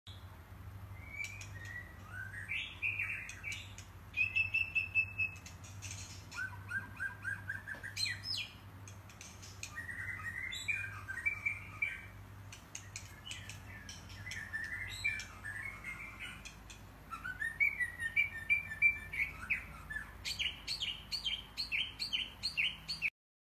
Bored Cats Watch Video Of Bird On Laptop
This is the sweet moment of two pet cats lay next to each other while watching a video of a bird. The cats, named Leena and Anny, were filmed watching the bird video on a laptop on their owner's bed in Bangkok, Thailand, on November 6.  The owner said: ''There's no birds around here to keep them entertained, so showing bird videos in the laptop to my cats might be a good idea ''